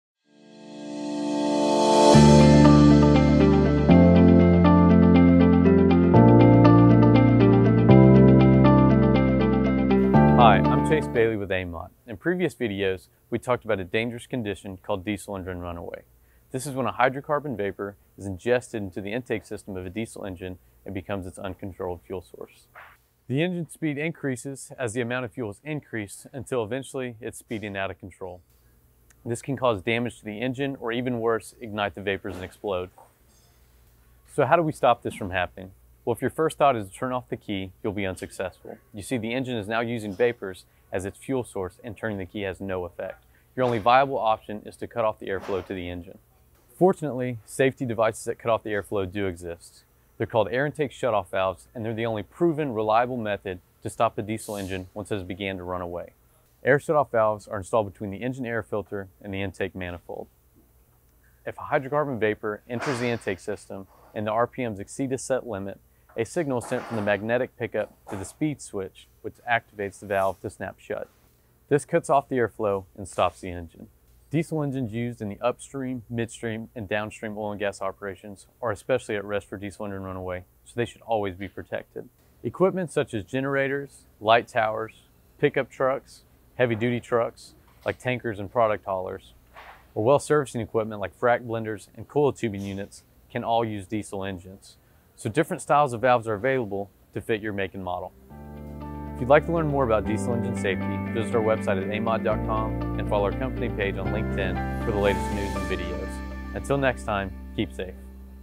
Ask the Expert: Preventing Diesel Engine Runaway
Diesel engine runaway is a dangerous condition in which a hydrocarbon vapor is ingested into the air intake system of a diesel engine, causing it to speed out of control. Consequences range from equipment damage to operator injury or death. Fortunately, safety devices called air intake shut off valves can prevent this from happening.

There are many different styles and sizes of valves available, so it’s easy to find one that will fit your engine make and model. Engines used in upstream, midstream, and downstream oil and gas operations are especially at risk of becoming an ignition source to an uncontrolled hydrocarbon release. They need to be protected. Examples of these types of engines include:
• Generators
• Light towers
• Well servicing equipment, like frac blenders and coil tubing units
• Pickup trucks
• And heavy-duty trucks, like tankers and product haulers

to view the entire selection of air shut off valves.